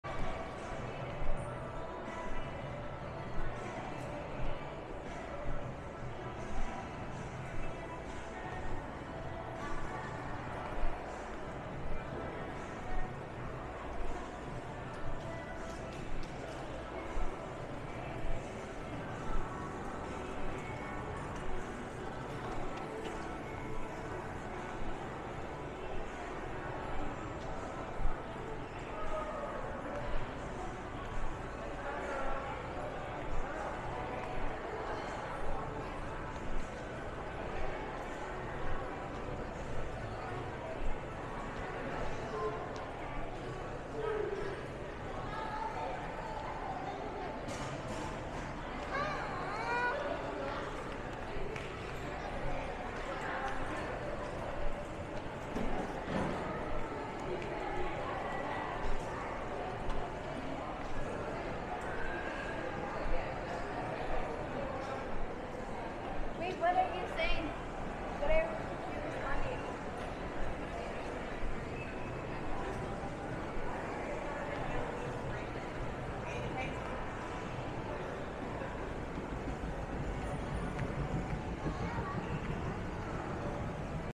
San Diego Shopping : Parkway Plaza Shopping Mall - Part 2
Walking Parkway Plaza Shopping Mall in San Diego California - Part 2